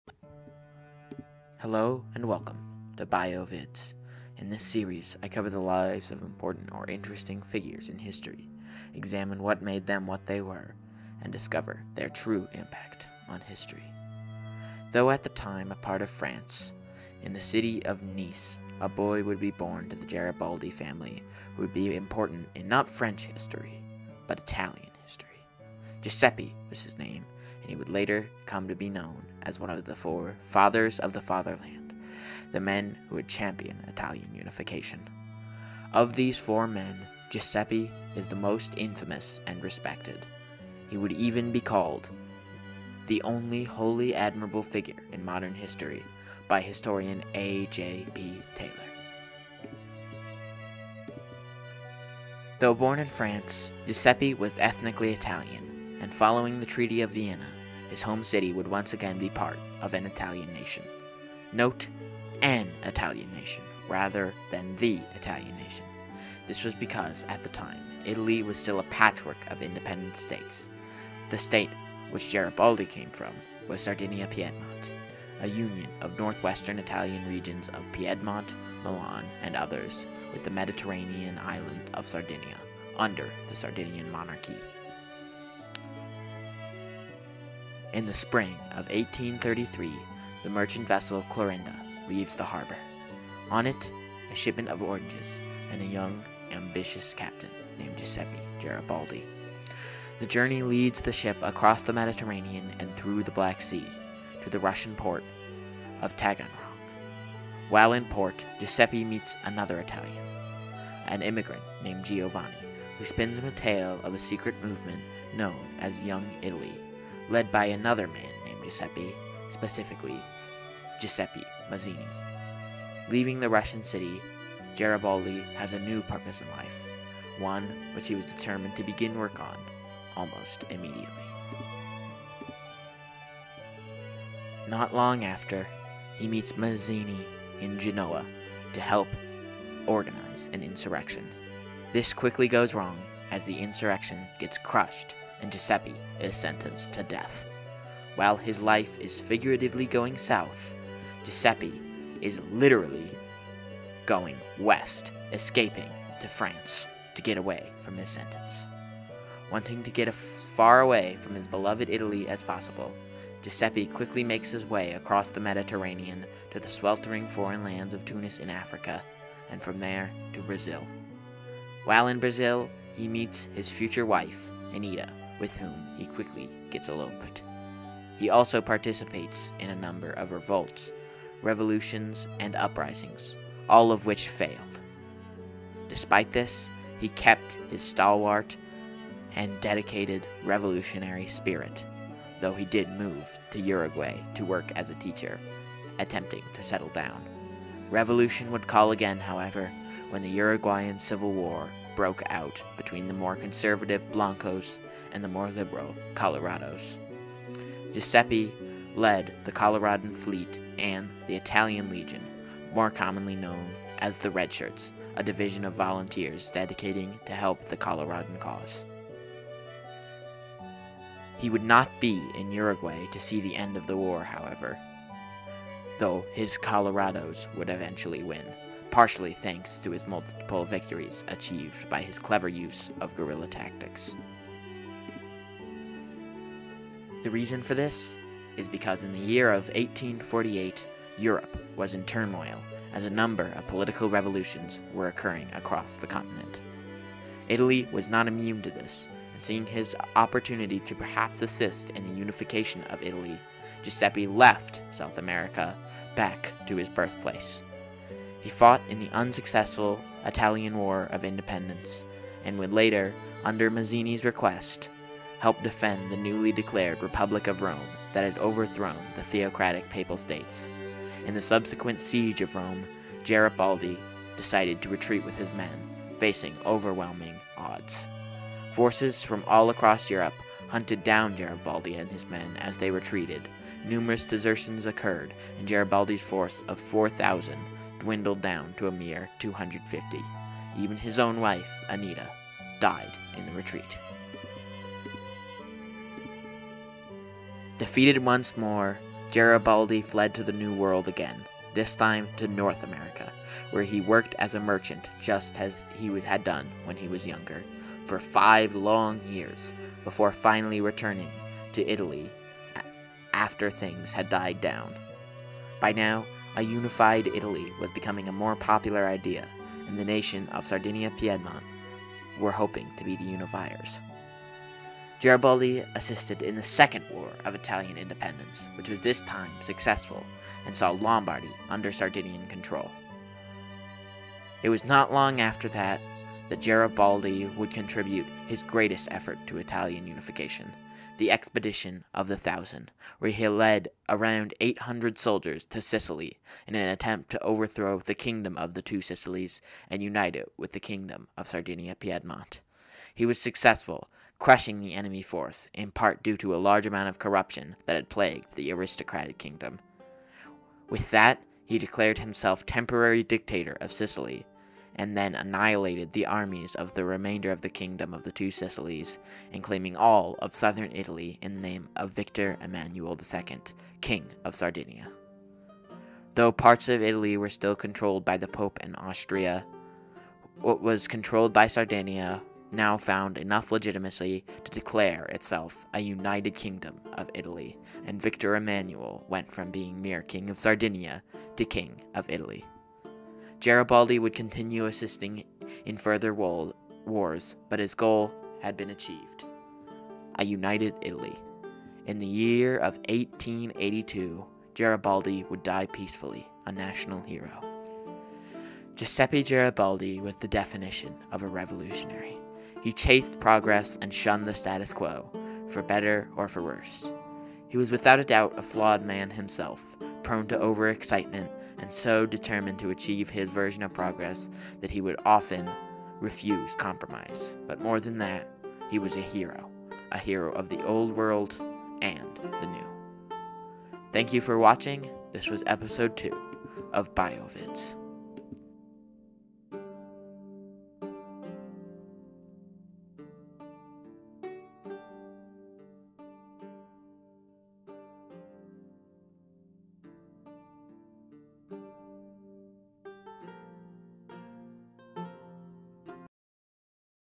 Giuseppe Garibaldi - BIOVIDS
Biovids Ep 2 - Giuseppe Garibaldi

In this video I cover the life and impact of Italian patriot Giuseppe Garibaldi, best known for helping unify Italy into a single country.

0:00 Intro
0:52 Early Life
2:39 Life as a Freedom Fighter
5:34 Final Reunification
6:48 Outro